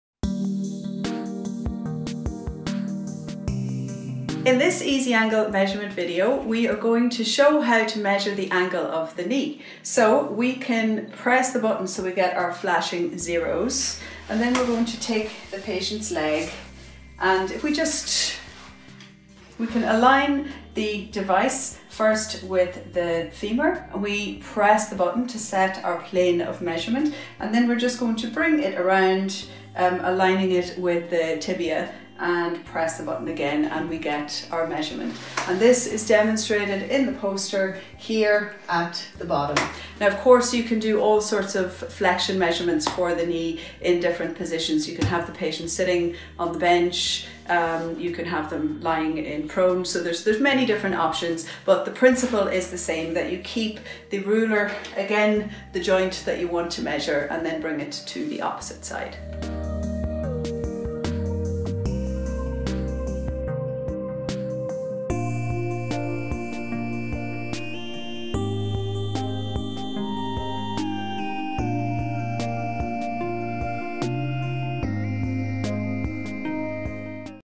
EasyAngle - Measuring Knee Flexion
In this EasyAngle measurement video, we demonstrate how to measure Knee Flexion using the EasyAngle digital goniometer.

The EasyAngle performs the functions of the goniometer, inclinometer, scoliometer,  BROM and CROM.